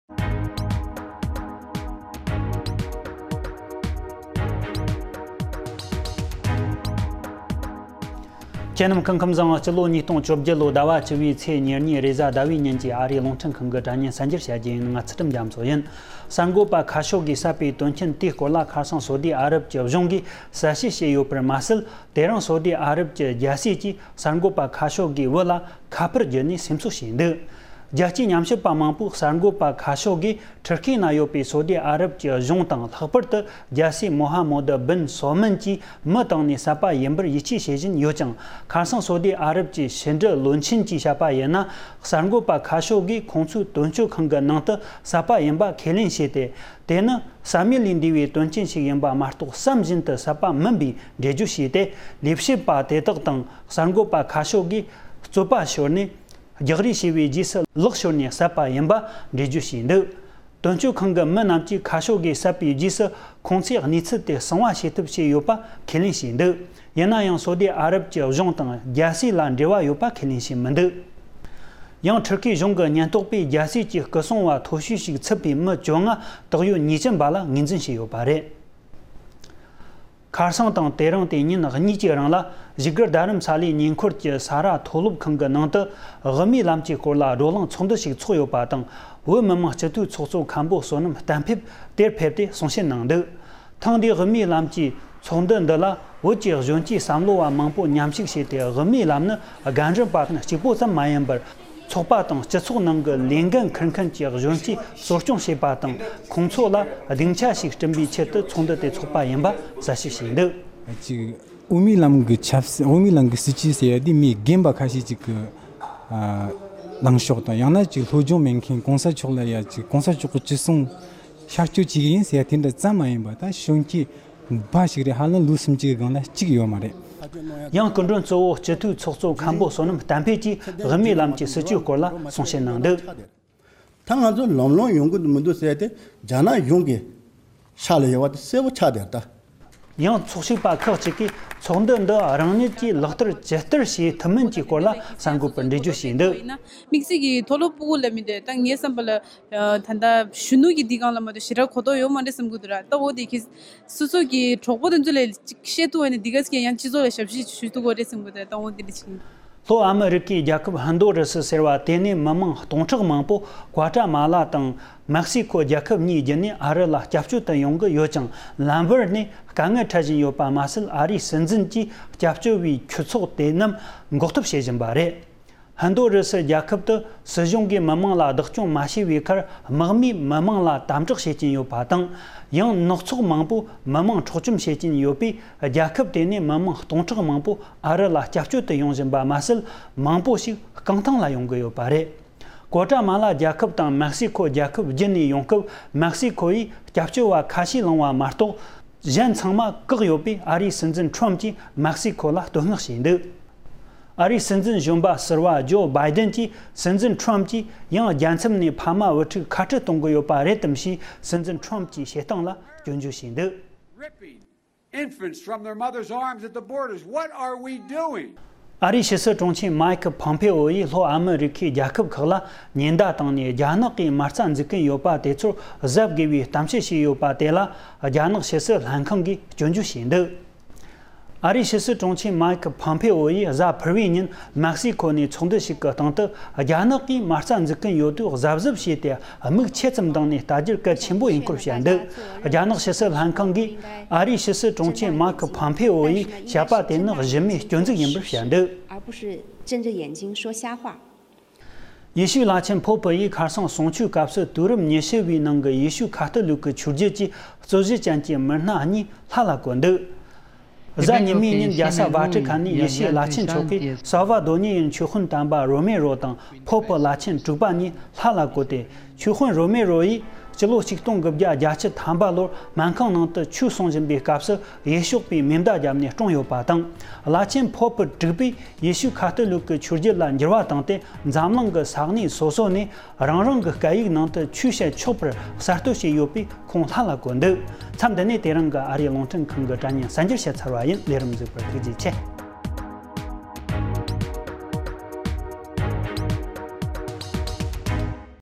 Headline News Oct 22, 2018 དྲ་བརྙན་གསར་འགྱུར། ༢༠༡༨ ཟླ་ ༡༠ ཚེས་༢༢
གསར་འགོད་པ་བསད་པར་གཞུང་གིས་གསལ་བཤད།
དབུ་མའི་ལམ་གྱི་བགྲོ་གླེང་ཚོགས་འདུ།
སྐྱབས་བཅོལ་བའི་ཁྱུ་ཚོགས་བཀག་པ།
ཨ་རིའི་ཕྱི་སྲིད་དྲུང་ཆེ་ལ་སྐྱོན་བརྗོད།
ཡེ་ཤུའི་བླ་ཆེན་གྱིས་མི་གཉིས་སྐྱེས་ཆེན་ལ་ངོས་འཛིན།